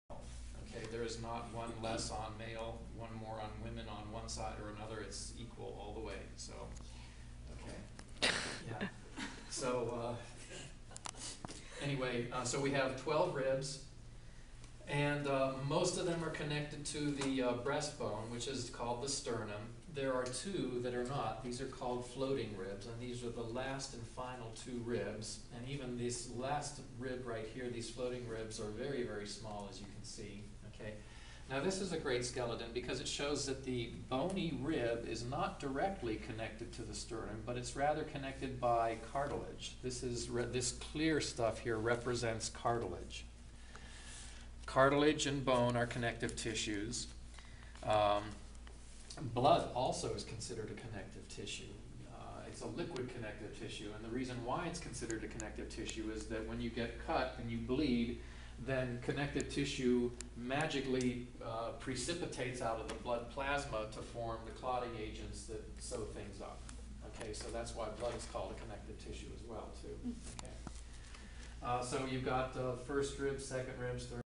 Yoga Teacher Training with Laurel Hodory
Guest teacher instructs yoga anatomy.